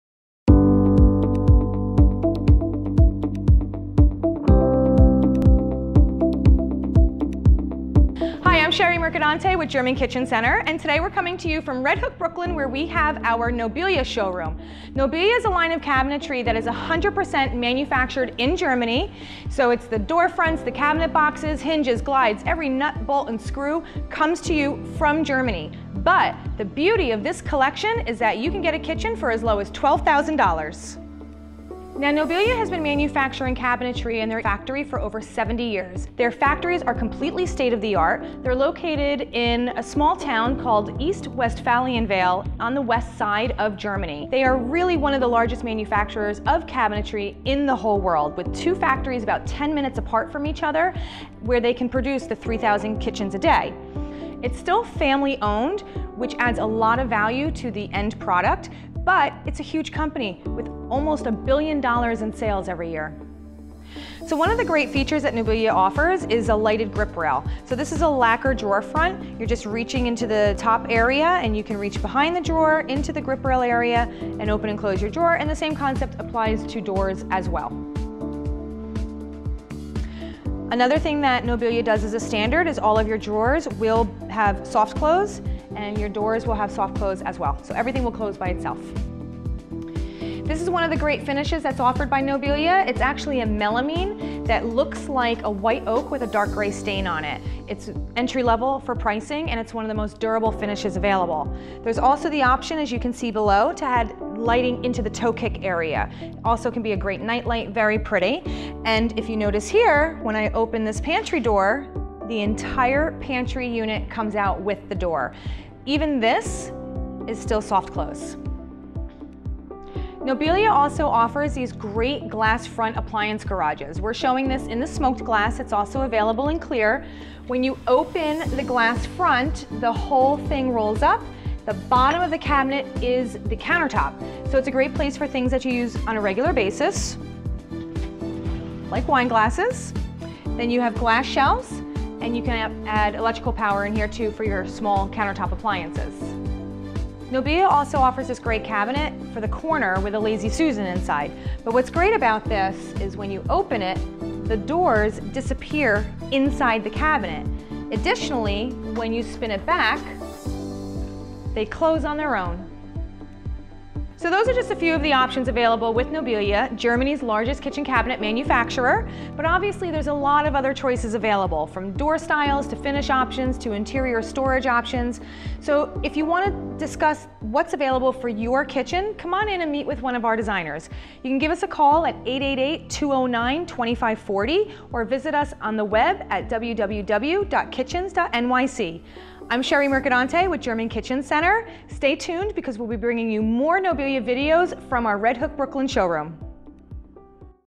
About Nobilia: Europe's Most Popular Kitchen Brand
Get a glimpse inside our Red Hook, Brooklyn showroom which features the top kitchen manufacturer from Germany and all of Europe, Nobilia. Marketing Director Sheri Mercadante provides a little history into the company and some reasons why so many people today are choosing a custom Nobilia kitchen. Also get an inside look a few select kitchens that demonstrate Nobilia's innovation and versatility.